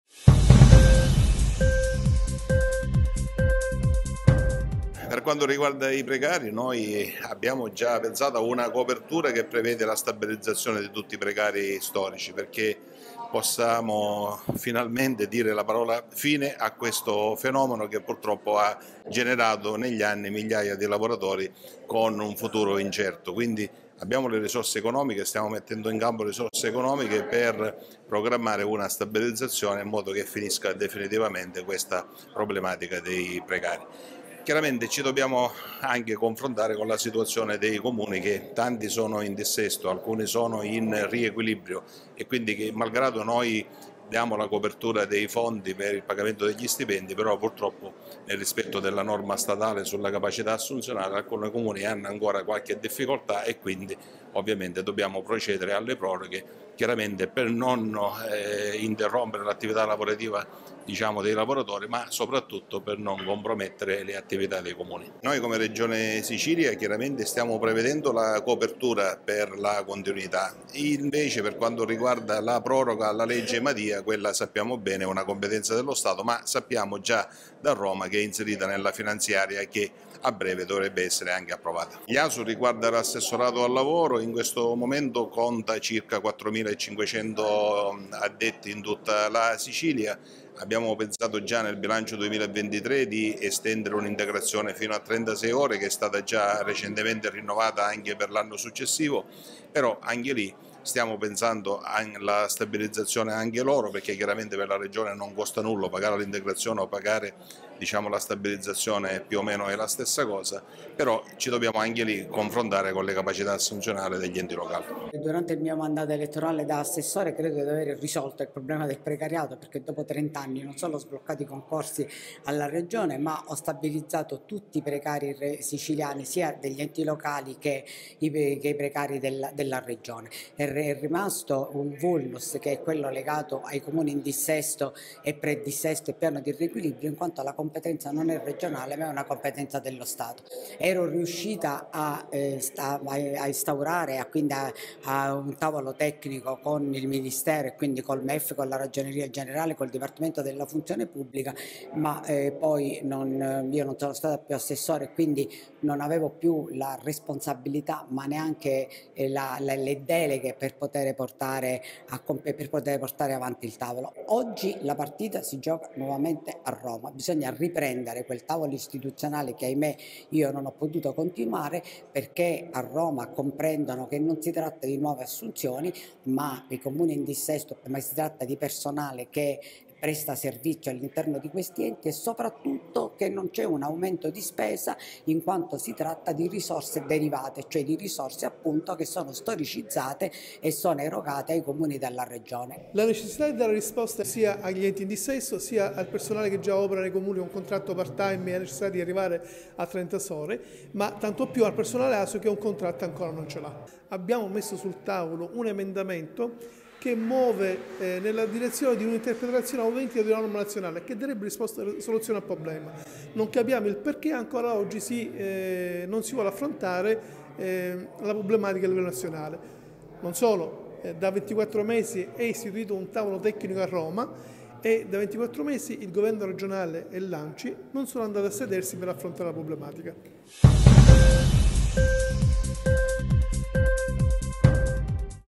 Precari degli enti locali, verso una nuova proroga sui contratti in scadenza al 31 dicembre
Sul tavolo il futuro di 1500 contrattisti e 4500 Asu siciliani rimasti ancora nel limbo senza stabilizzazione. Il punto in un’assemblea ad Acquedolci